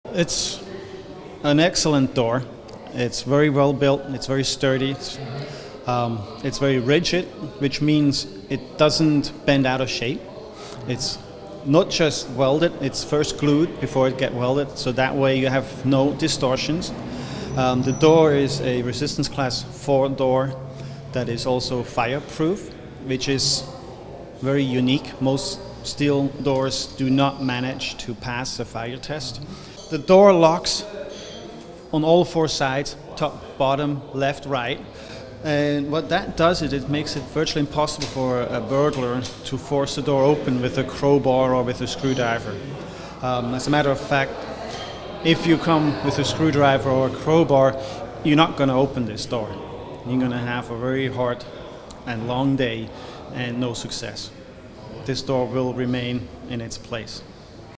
Security Doors ADLO, excellent door
It is an excellent door, it's very well built; it's very sturdy, it's very rigid, which means it doesn't bend out of shape; it's not just welded but it's first glued before it gets welded so that way you have no distortions.